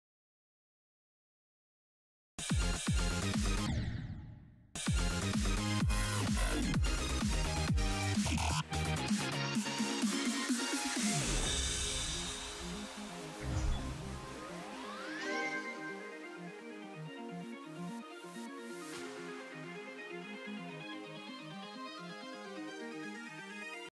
L A G
i'm making my own version of this level, since the original is buggy and the difficulty isn't very consistent, so I want to make it fun.